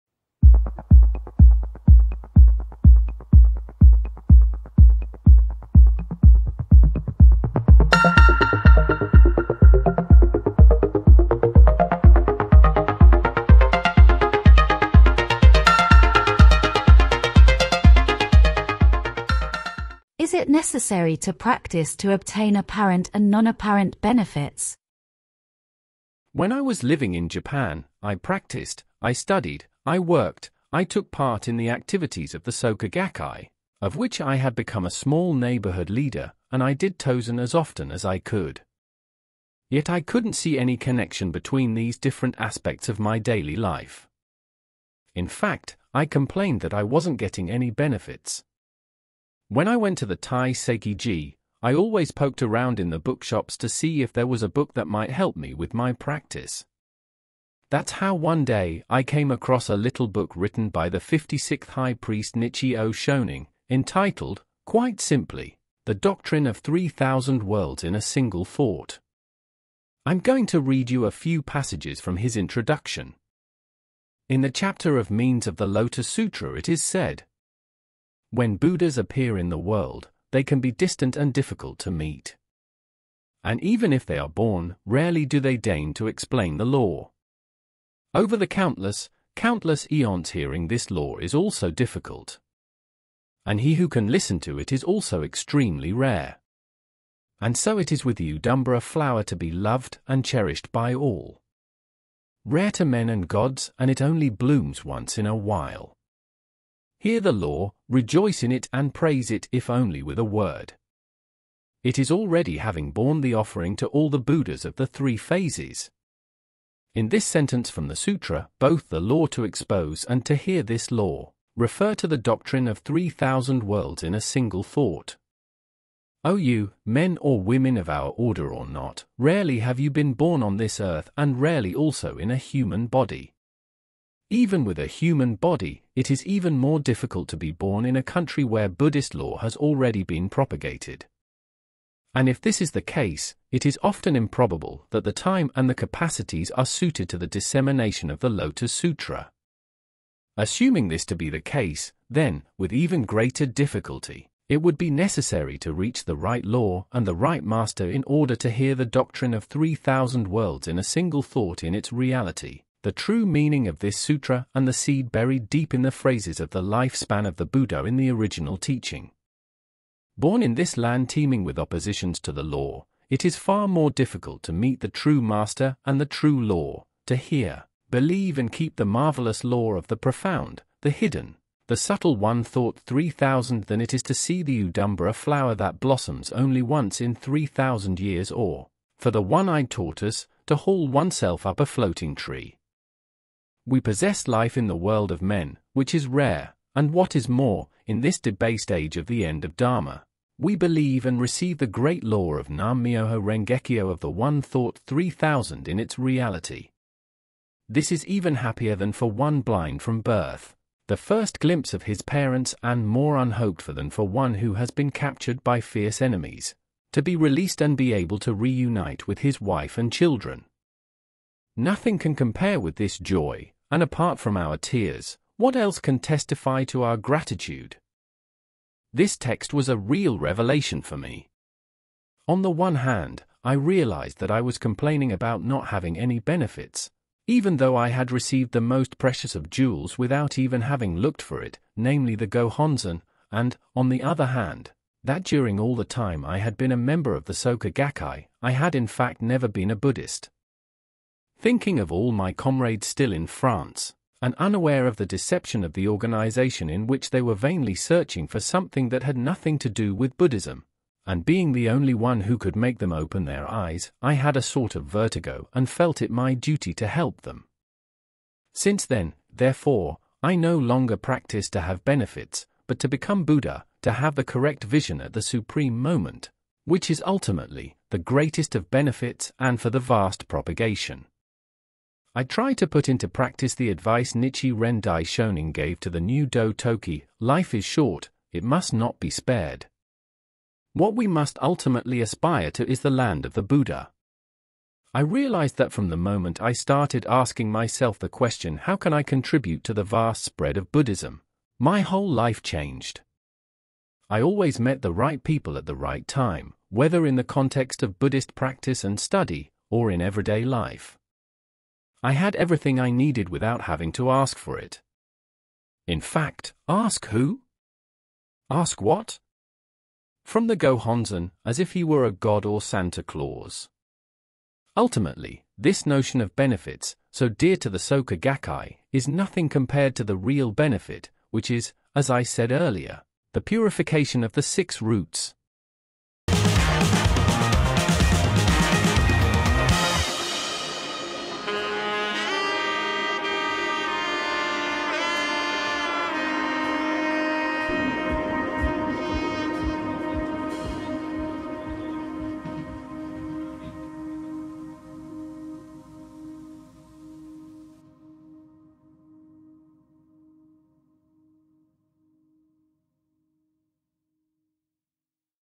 Question n° 12 - Is it necessary to practise to obtain apparent and non-apparent benefits?
What, ultimately, is the purpose of practising Buddhism? This podcast answers that question.